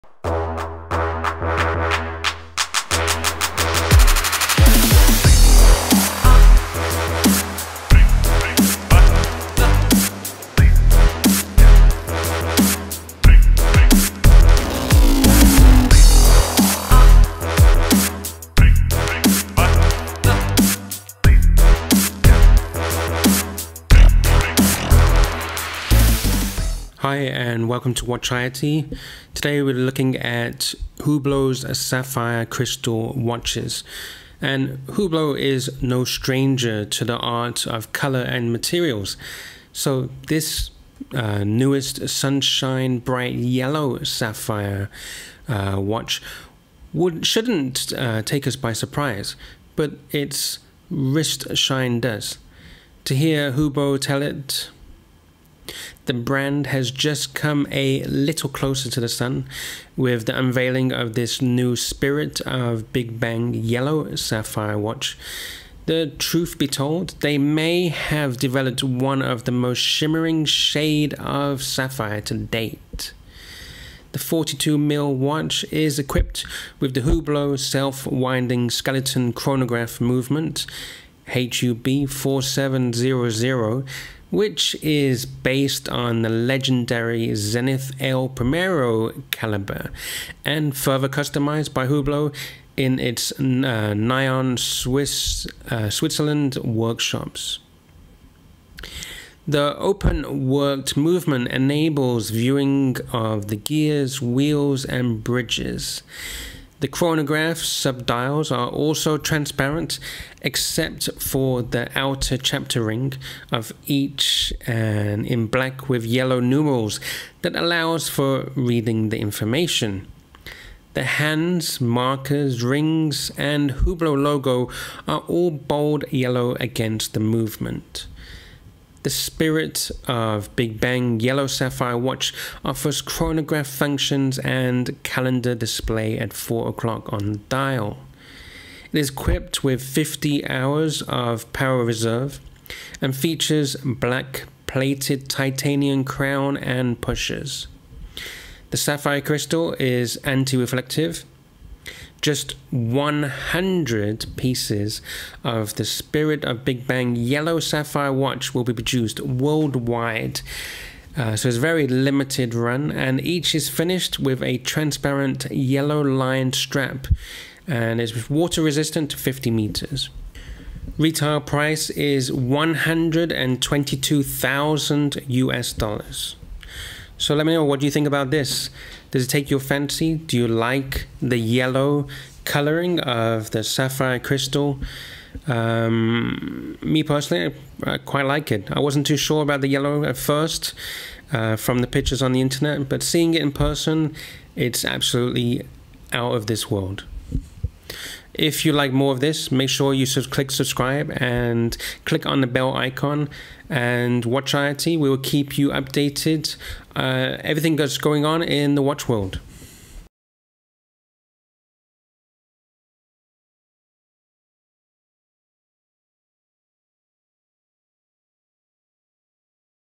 HUBLOT Yellow Sapphire Spirit of Big Bang Watch (LIMITED TO 100) - Review Hands On
Hublot is no stranger to the art of colour and materials, so this newest sunshine bright yellow sapphire watch shouldn’t take us by surprise, but its wrist shine does. To hear Hublot tell it, the brand has just come “A little closer to the sun” with the unveiling of this new Spirit of Big Bang Yellow Sapphire watch. The truth be told, they may have developed one of the most shimmering shade of sapphire to date.

Hublot is no newcomer to the sapphire case craze that has been taking a handful of brands by storm. In fact, it was more than three years ago that Hublot unveiled its first sapphire cased watches. The typically transparent materials is ultra-resistant to breakage and, as such, is quite difficult to machine. The making of a sapphire case takes hundreds of hours of milling, and finishing and polishing. Once Hublot had mastered the art, the brand wasn’t happy to rest on its laurels.

A year later, in 2017, Hublot unveiled its first sapphire cased watches in colour – introducing hues of smoked black, blue and red (more of a pink). Now, after even more time spent in research and development, Hublot unveils the boldly beautiful primary colour yellow in a sapphire case.

COMPLETELY TRANSPARENT
In search of the perfect fusion between movement and case, Hublot engineers and chemists have researched and developed a transparent material which reveals the heart of the watch, all while being robust enough to effectively protect the mechanism. That’s how the Manufacture has become the expert of synthetic sapphire, a material from which it derives its esthetic qualities that are unique within watchmaking – transforming each watch into a work of art. transparent blue yellow red pink green mp-11 la ferrari archieluxury

In addition to the sapphire crystal-cased version of the MP-05 La Ferrari, the big “transparent” news from Hublot for 2016 is this all-sapphire-cased version of the Big Bang UNICO. Difficult to properly capture in pictures, I think anyone handling this interesting sapphire watch will be impressed by it, and compared to other all-sapphire crystal watches, the price is a relative bargain at under $60,000. In fact, the Hublot Big Bang UNICO Sapphire reference 411.JX.4802.RT might be the most affordable all-sapphire-cased watch to date.